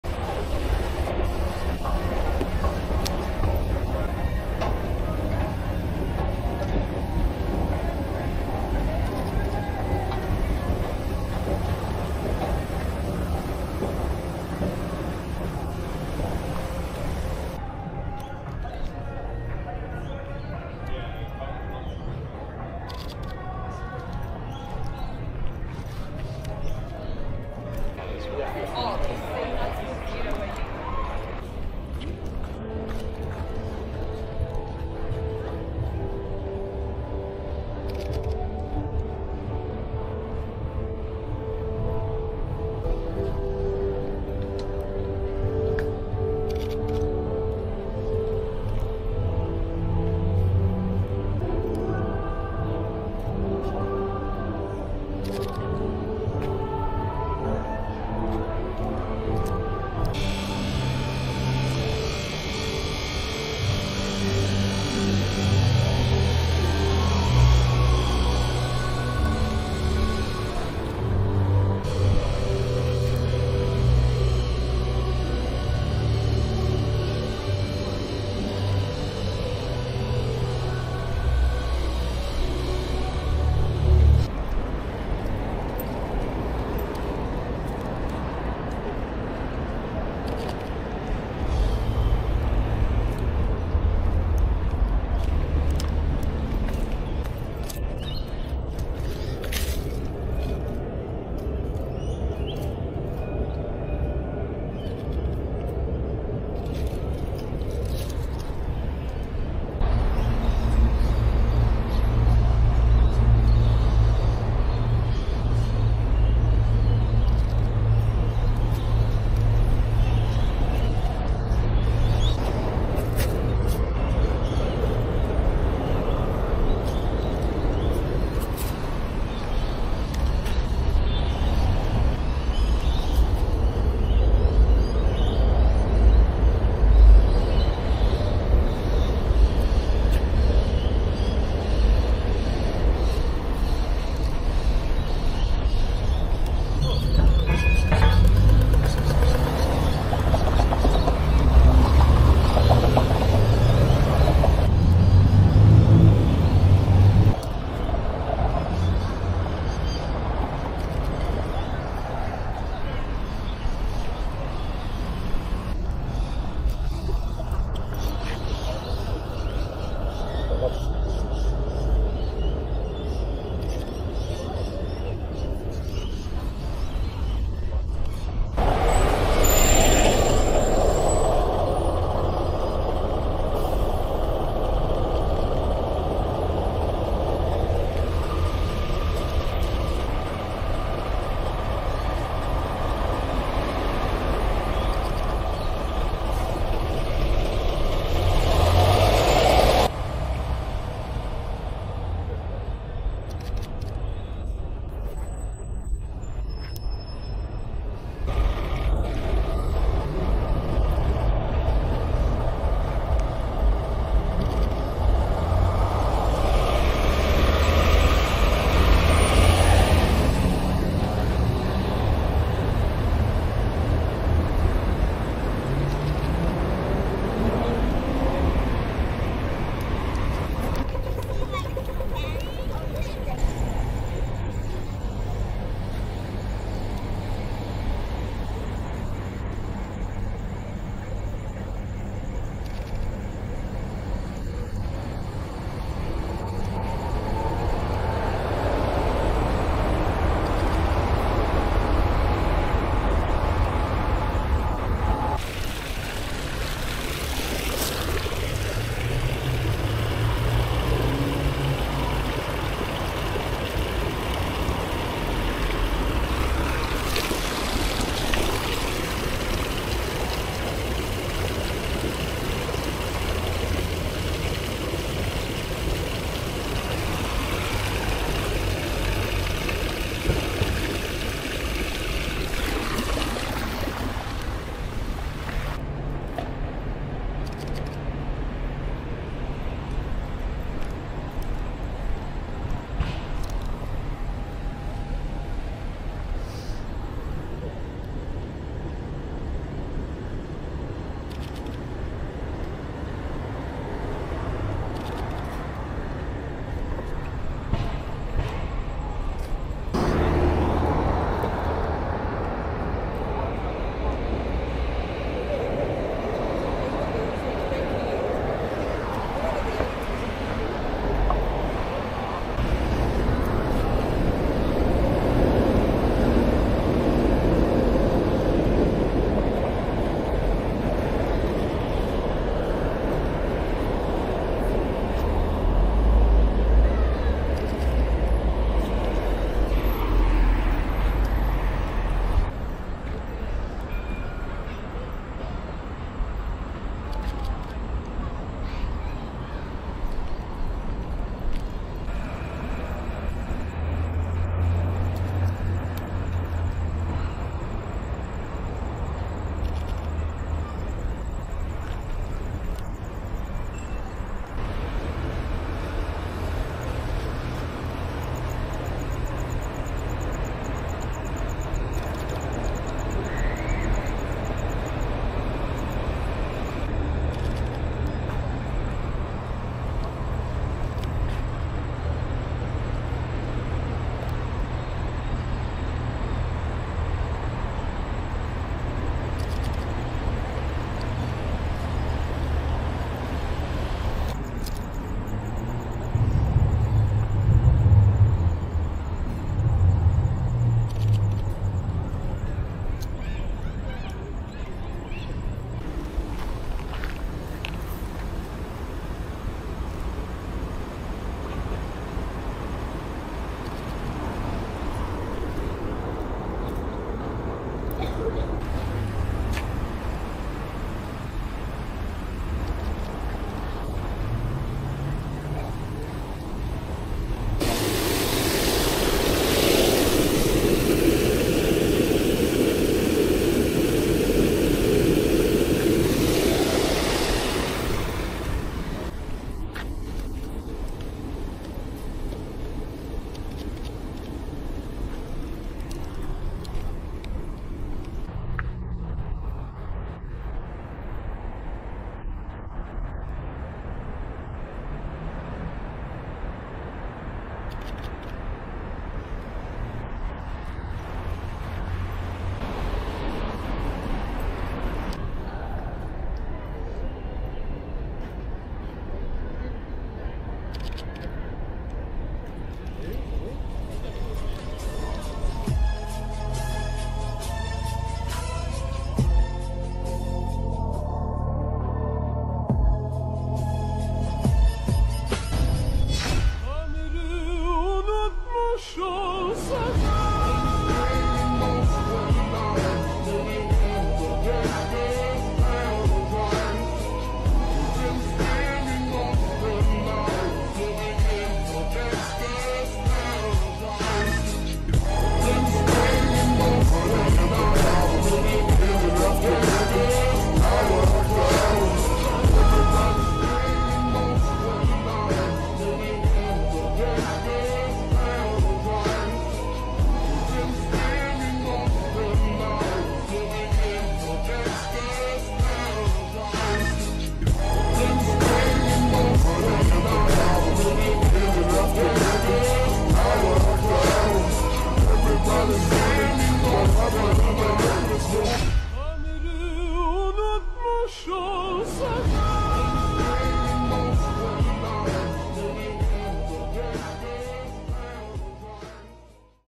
POV Street Photography - Adelaide At Night || Part 2
My second time taking photos at the city, this is part 2 of a 3 part 'series' (about 20 minutes worth). I thought you guys would rather be given three separate videos, than one long video... Let me know what you think!